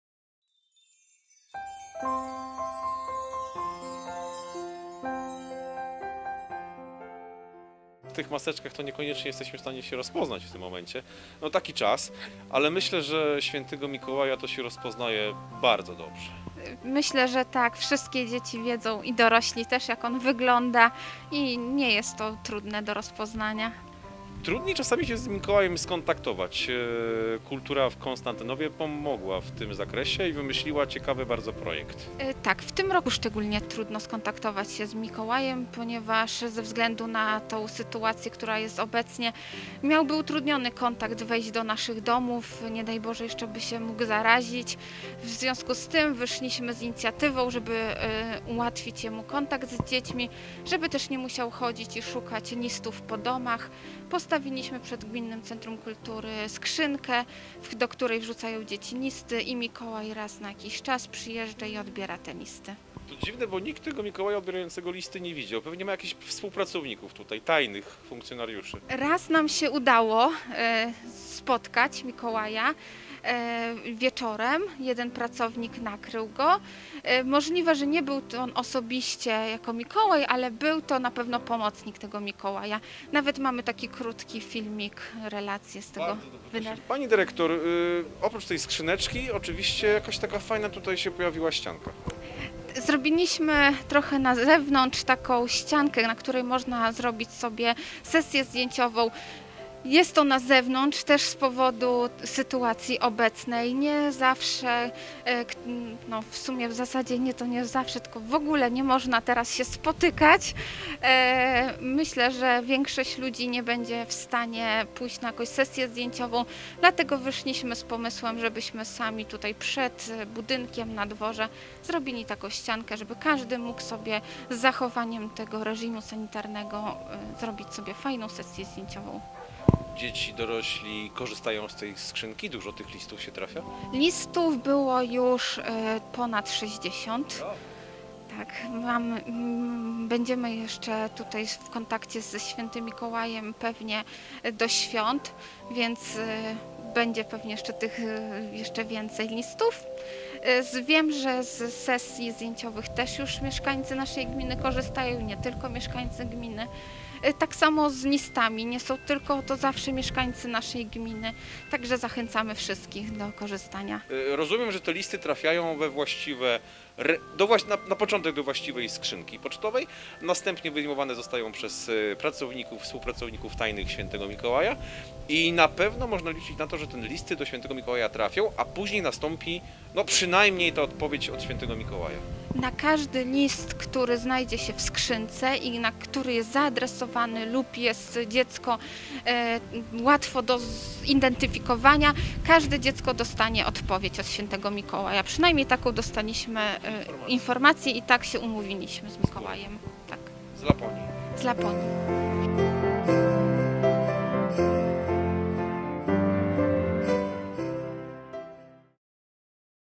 Napisz list do św. Mikołaja i zostaw w skrzynce przy GCK w Konstantynowie
Gminne Centrum Kultury w Konstantynowie wpadło na wspaniały pomysł, umieściło przed wejściem do budynku skrzynkę na listy do św. Mikołaja. Wszystkie listy są przekazywane Mikołajowi a mało tego każdy nadawca listu otrzyma odpowiedź od Mikołaja! :)